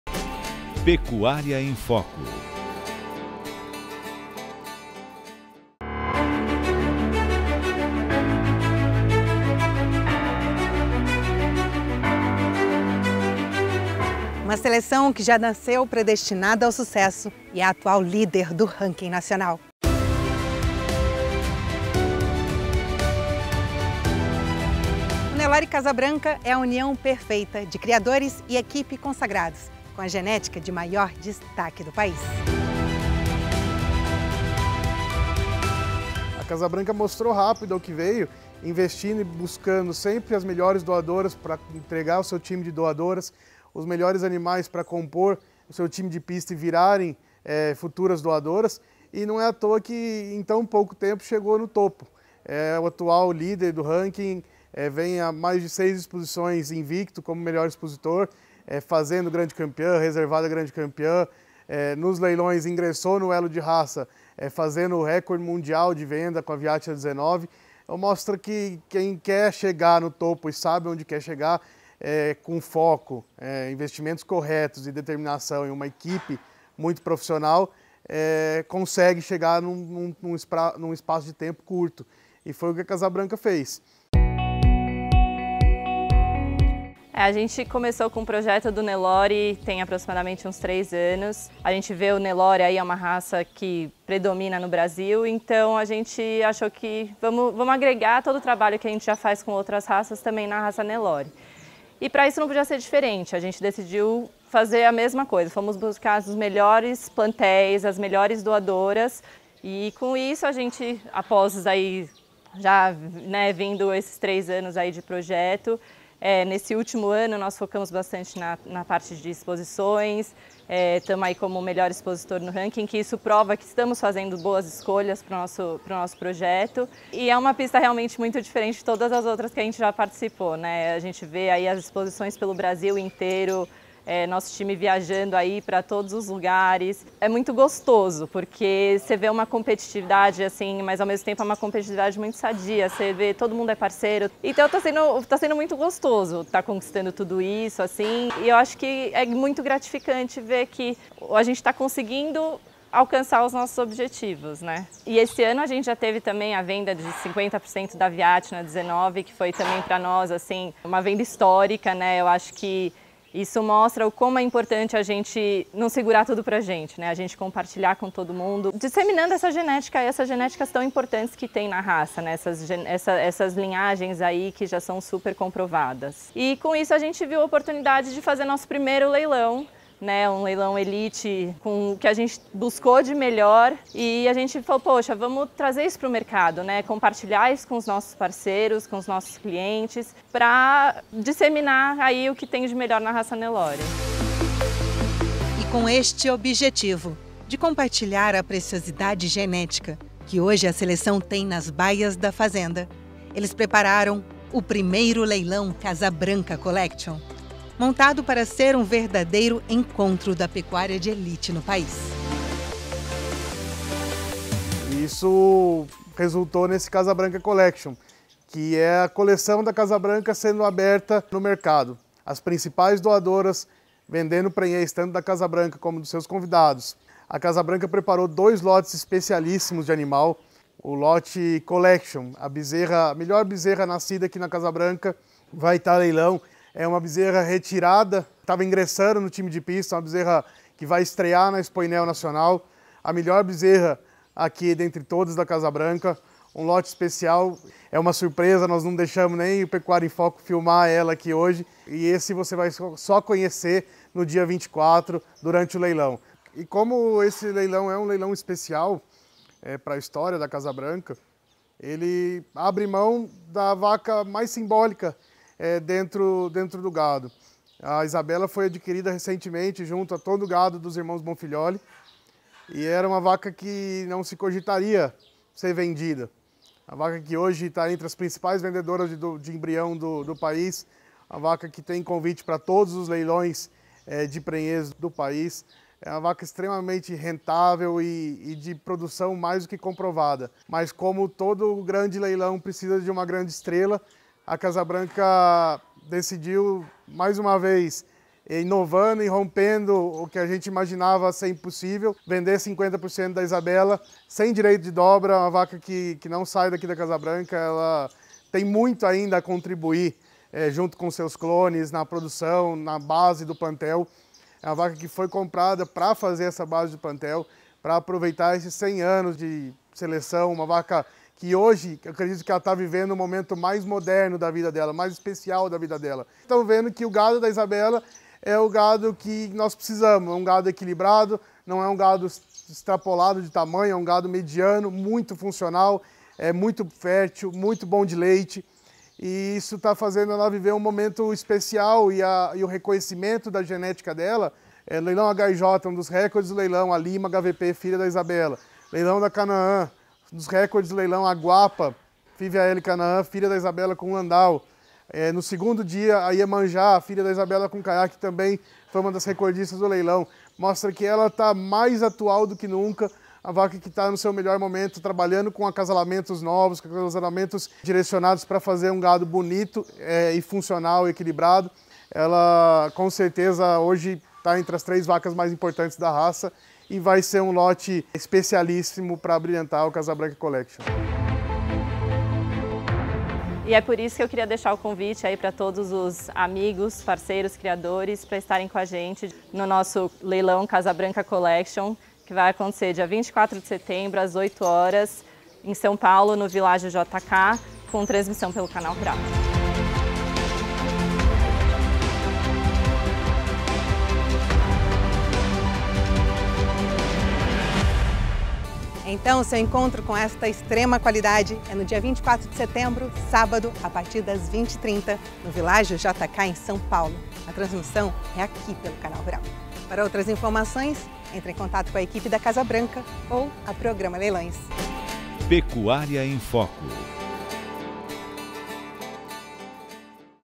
PECUÁRIA EM FOCO - LEILÃO CASA BRANCA COLLECTION NELORE
Uma seleção que já nasceu predestinada ao sucesso e atual líder do rank nacional , Nelore Casa Branca e a união perfeita de criadores e equipe consagradas a genética de maior destaque do país. E com este objetivo de compartilhar a preciosidade  genética que hoje a seleção tem nas baias da fazenda eles prepararam o 1º Leilão Casa Branca Collection.